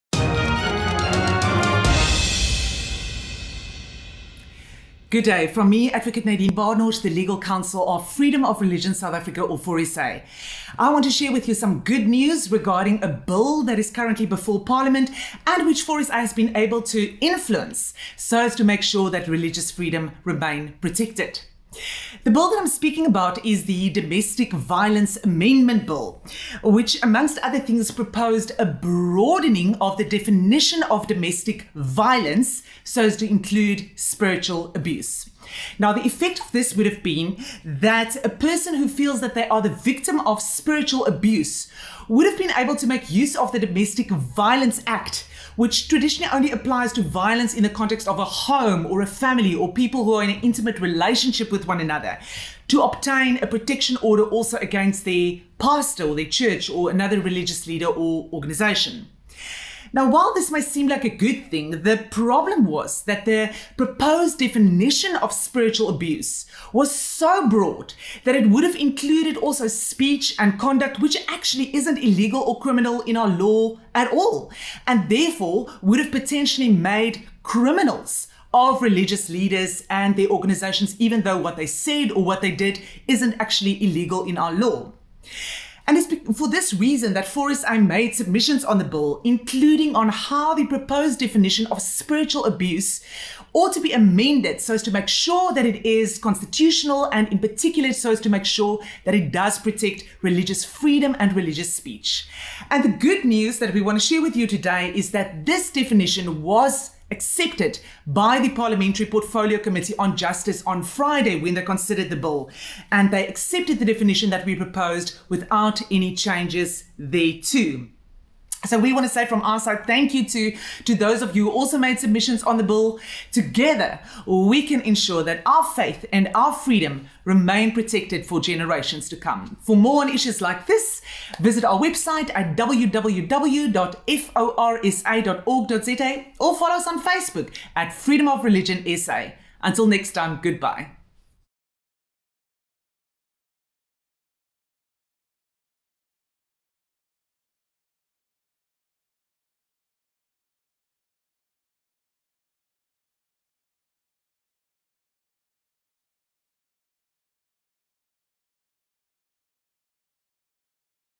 FOR SA - Religious freedom protected in Domestic Violence Amendment Bill
In this video, FOR SA's Legal Counsel Adv Nadene Badenhorst explains how Freedom of Religion South Africa (FOR SA) has been able to influence proposed amendments to the Domestic Violence Amendment Bill, and in particular the definition of "spiritual abuse", so as to make sure that religious freedom and religious speech remain protected.